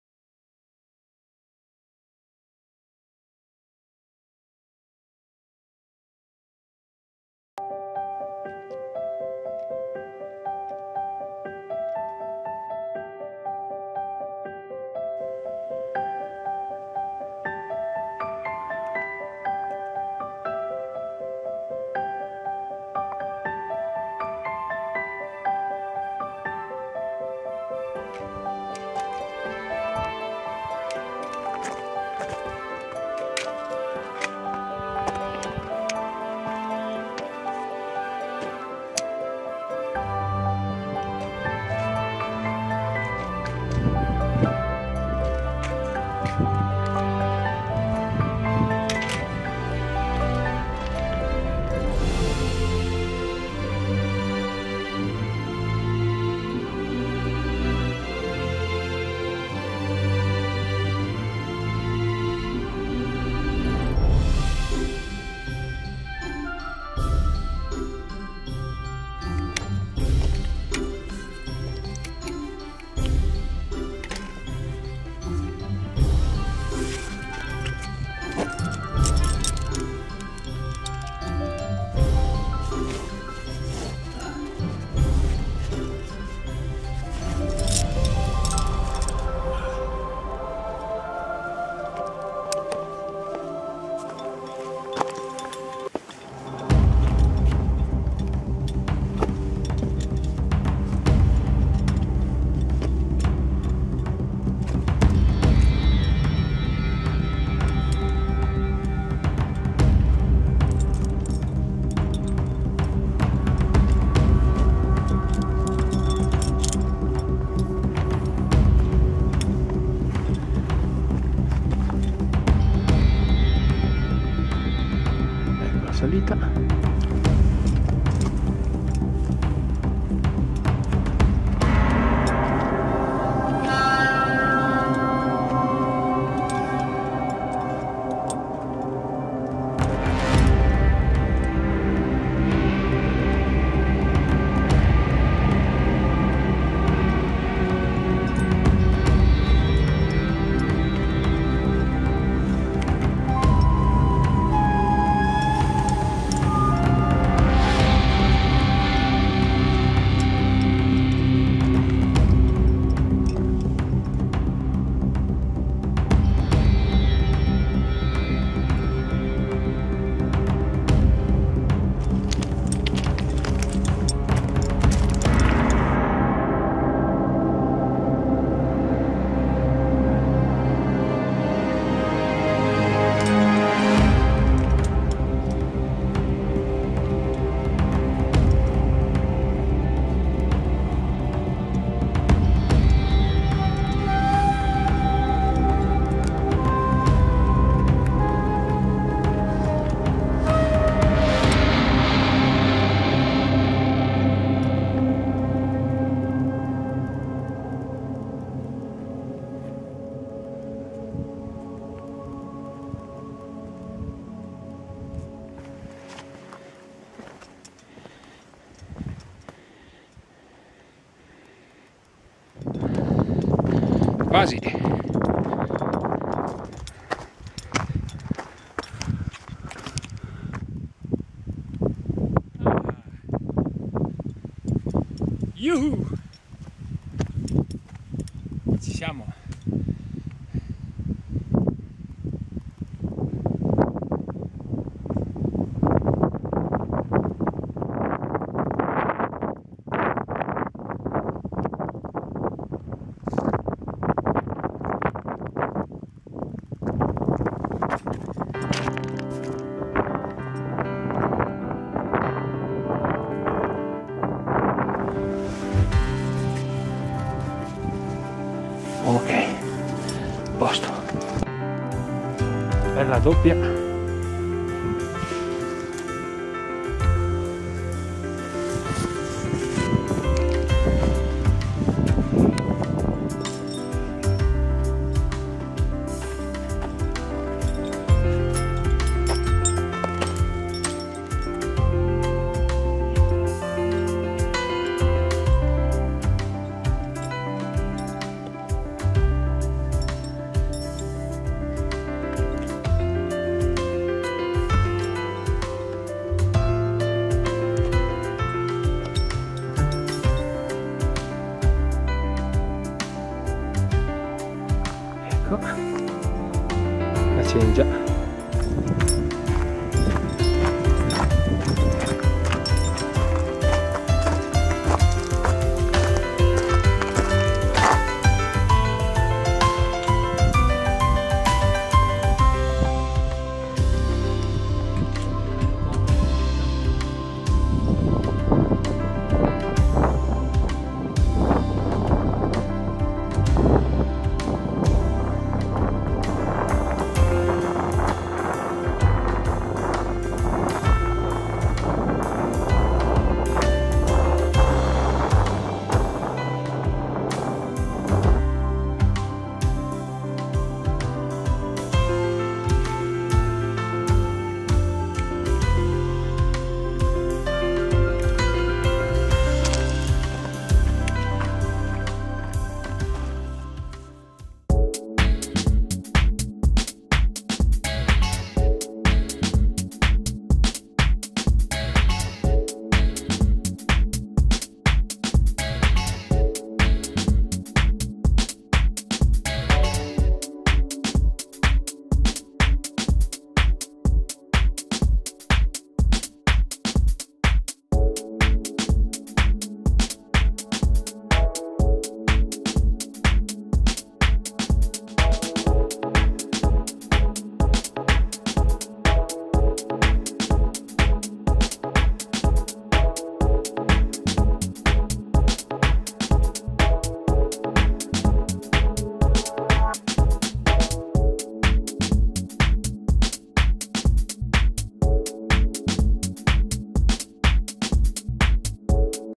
Cima Brenta, via normale da Sud
Salita alla cima Brenta, 3150 mt, per la parete Sud. Discesa per il versante orientale, fino alla cengia Garbari, poi per il sentiero attrezzato delle bocchette alte.